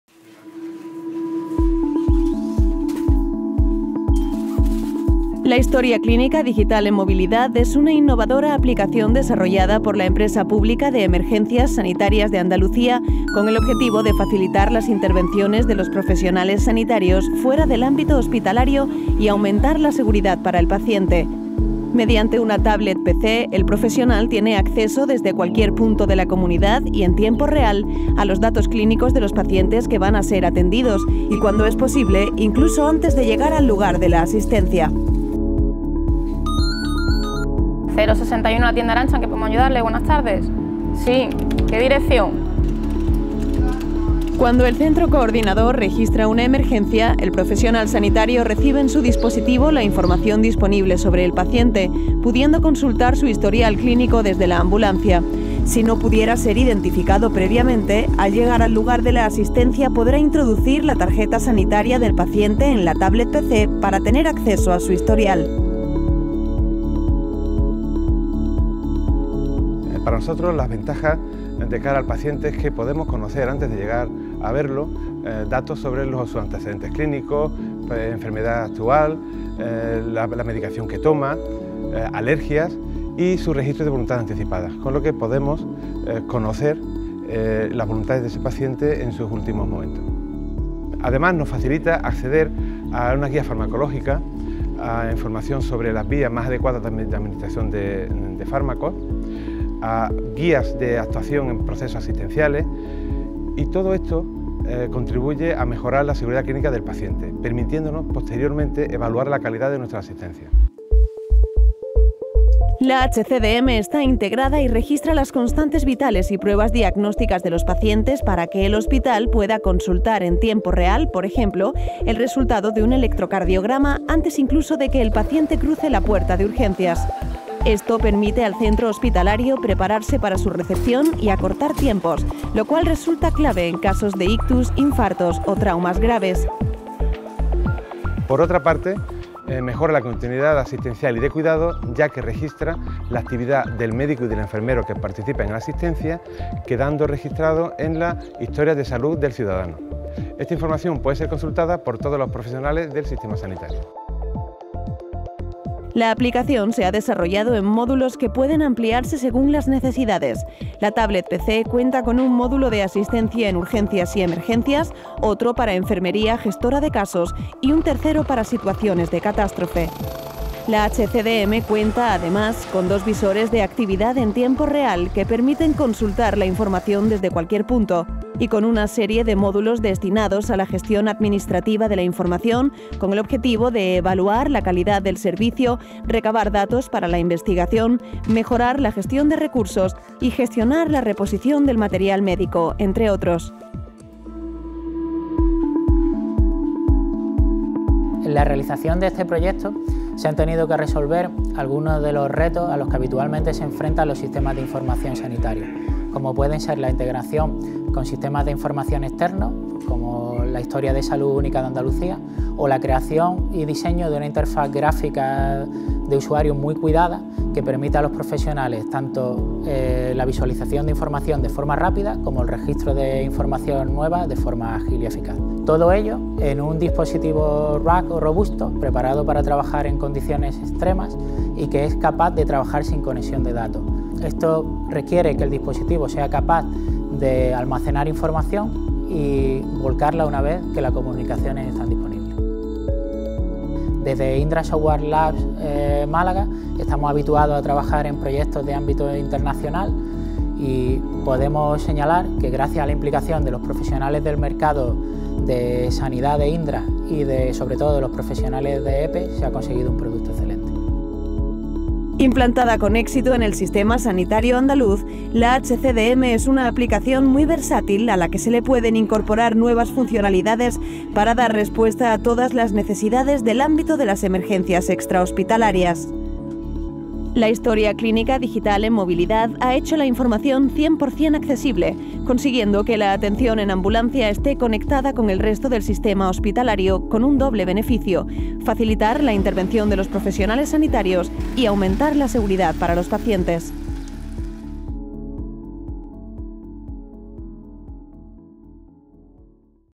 Historia Clínica Digital en Movilidad
Descripción del proyecto EPES-061 HCDM (Historia Clínica Digital en Movilidad), una solución de registro electrónico de pacientes para entornos de movilidad y emergencias.